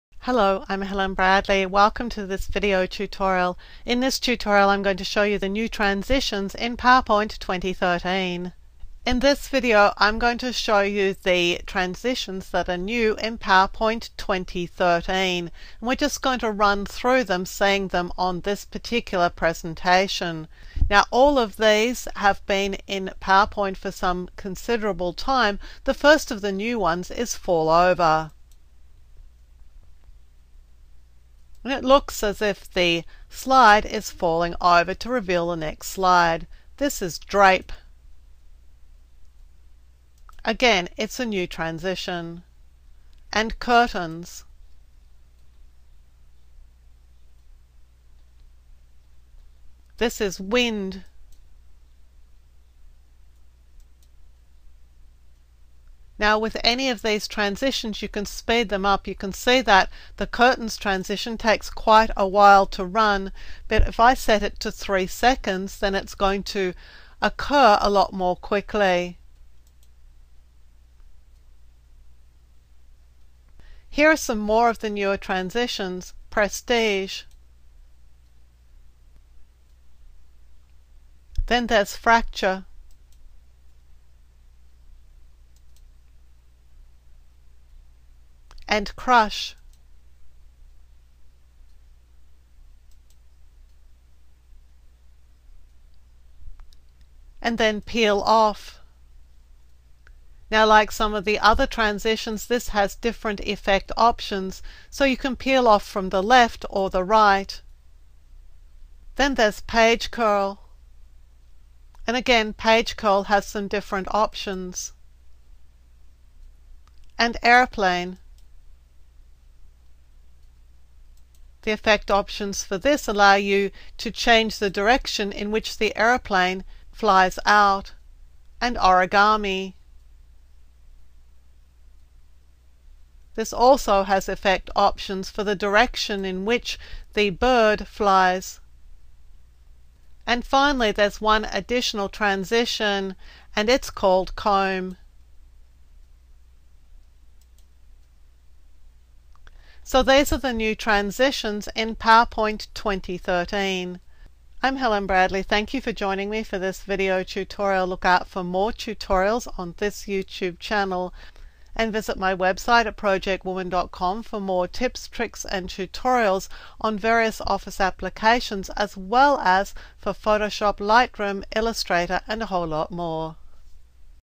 Learning PowerPoint 2013 -Mastering the New Transitions
See the new transitions in PowerPoint 2013 including airplane and origami. There are some great new transitions and this video shows you what they are, what they look like and some of the settings for them.

IF YOU LIKED THIS VIDEO YOU MAY ALSO LIKE THESE MICROSOFT OFFICE VIDEOS: